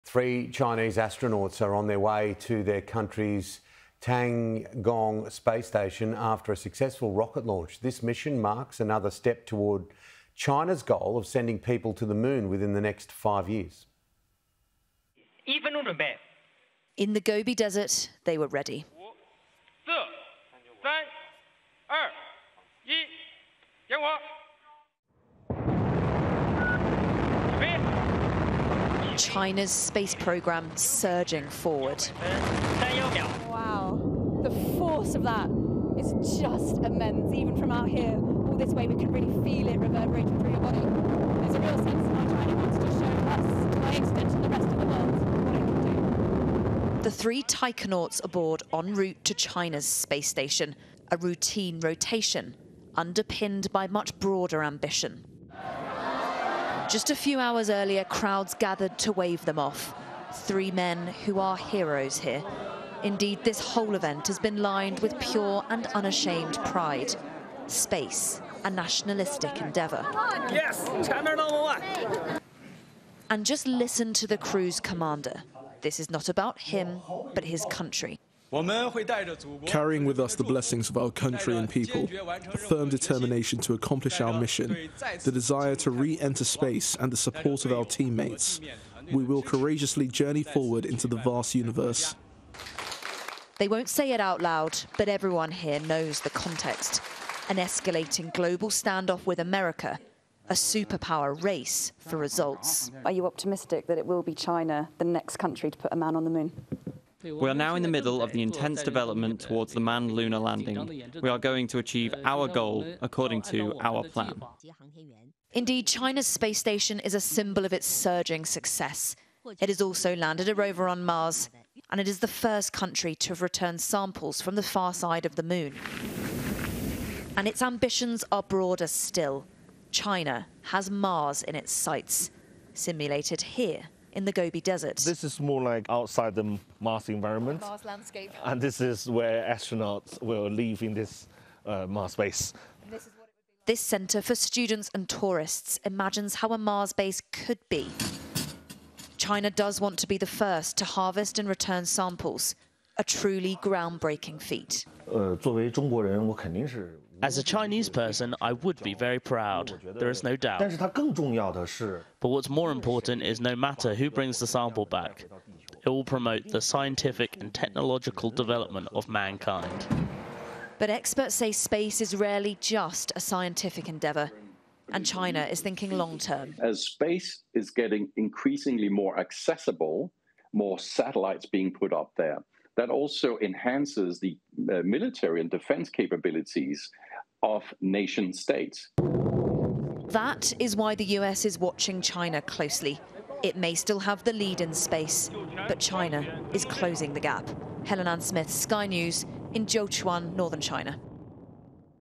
China launches Shenzhou-20 mission to Space
Three Chinese astronauts are on their way to their country's Tiangong Space Station after a successful rocket launch.

This mission marks another step toward China's goal of sending people to the moon within the next five years.